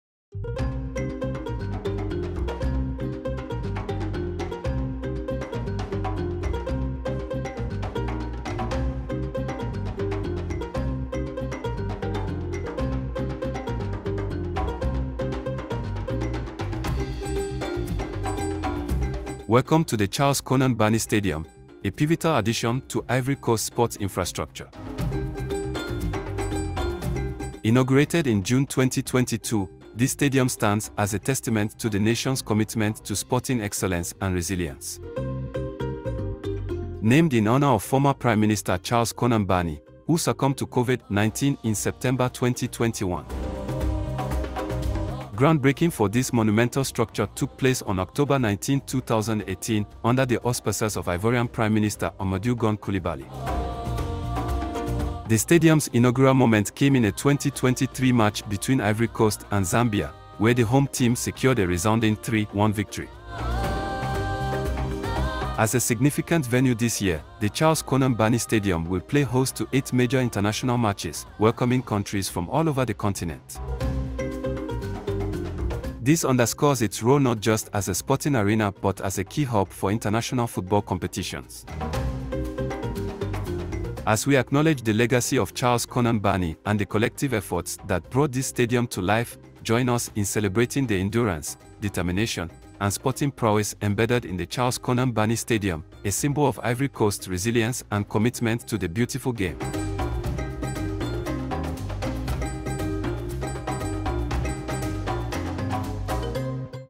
The STORY behind one of Ivory Coast's most FAMOUS stadiums 🏟️
Welcoming countries from all over Africa, the Charles Konan Banny Stadium played host to eight matches at this year's tournament.

A symbol of Ivory Coast's resilience and commitment to the beautiful game, the stadium was inaugurated in 2022 and named after former president, Charles Konan Banny.

Learn more about one of the country's most famous stadiums on this drone tour.